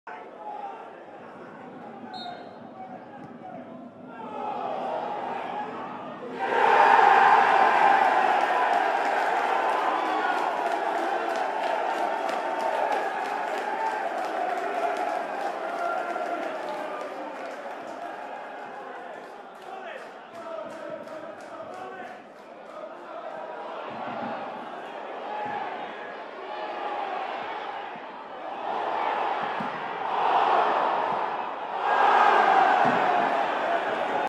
AFC Wimbledon defender's heroics at both ends of the pitch
Deji Oshilaja's vital contribution on Saturday